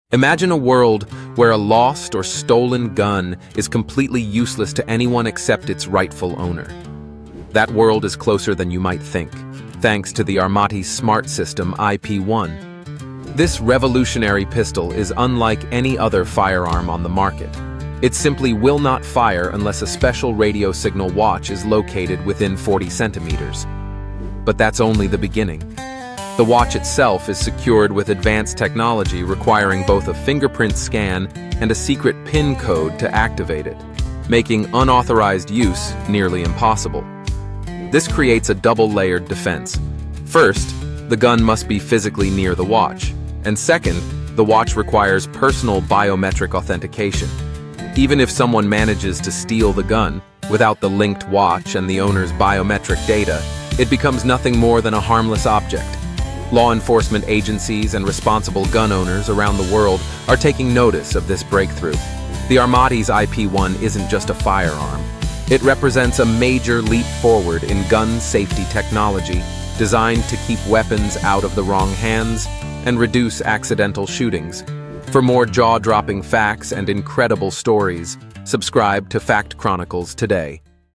This Gun Won’t Fire Without Its Owner’s Fingerprint
Meet the Armatis iP1: a pistol that only fires in the right hands. Discover how technology is reshaping firearm safety!